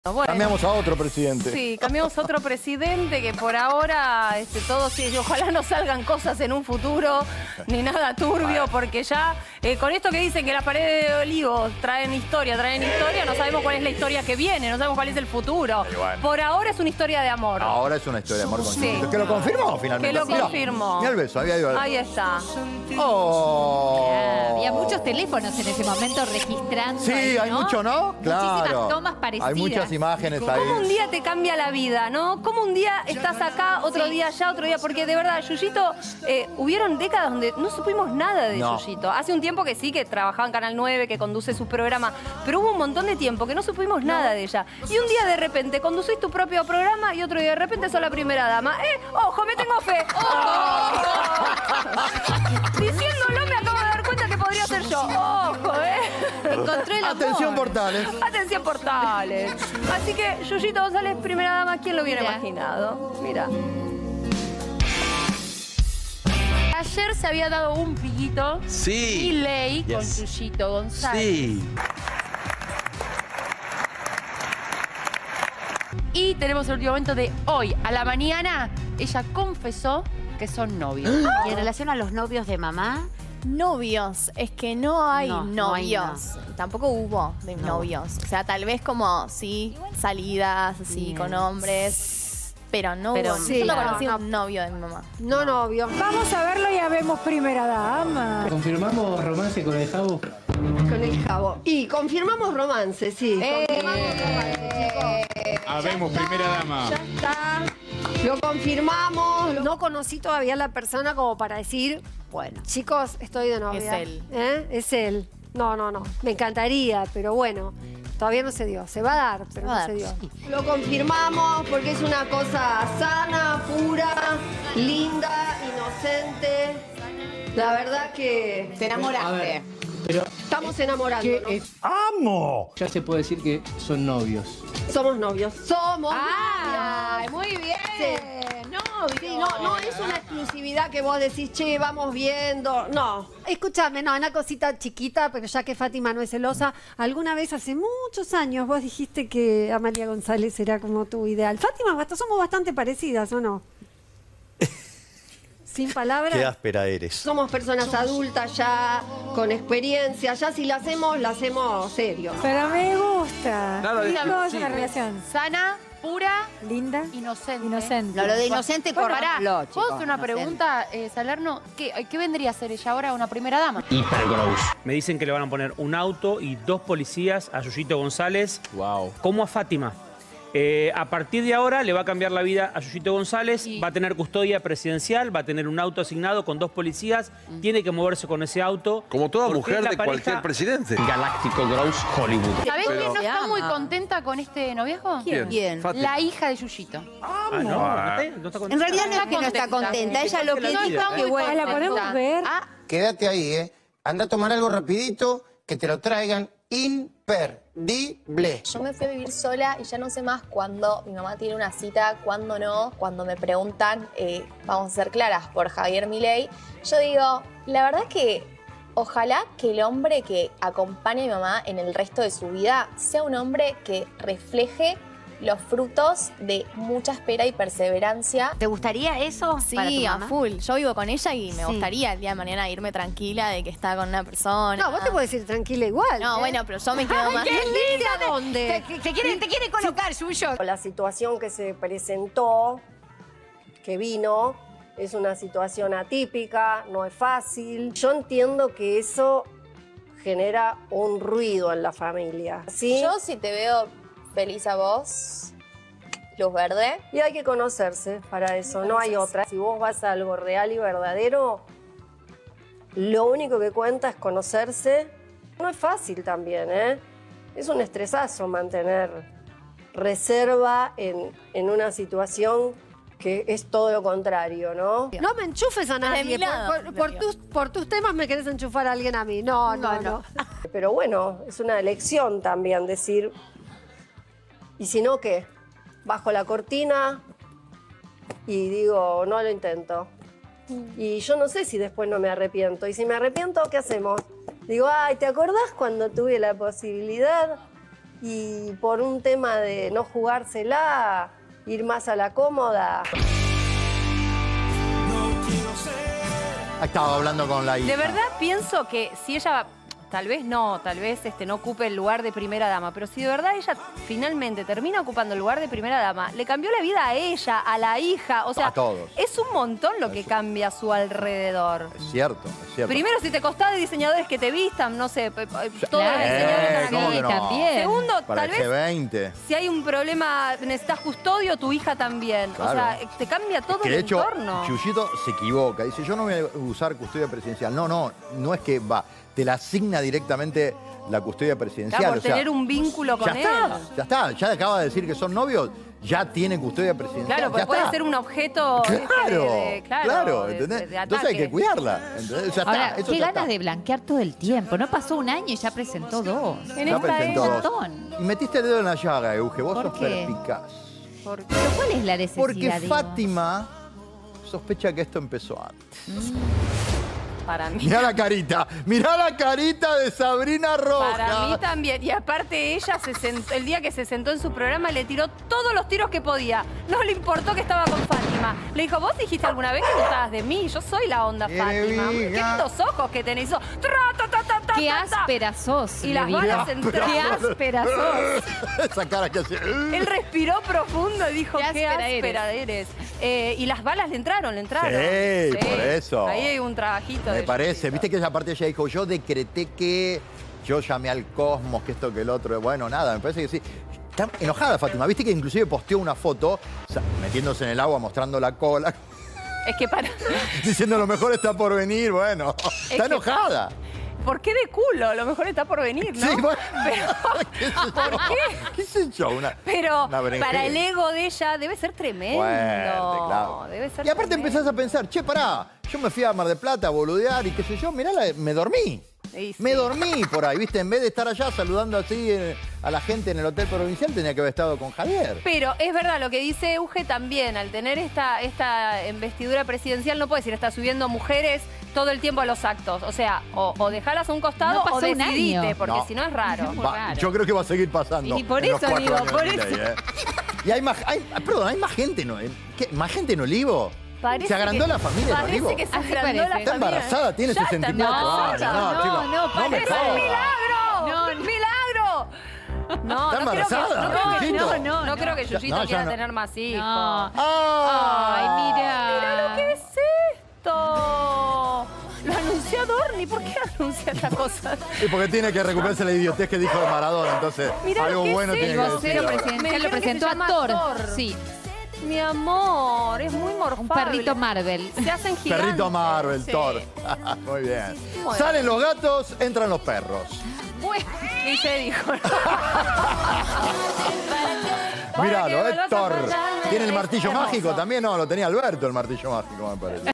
❤️ JAVIER MILEI y YUYITO GONZÁLEZ confirmaron su romance
Luego de darse un beso en público en el ex Centro Cultural Kirchner, la actriz dio a conocer la noticia en televisión: "Somos novios".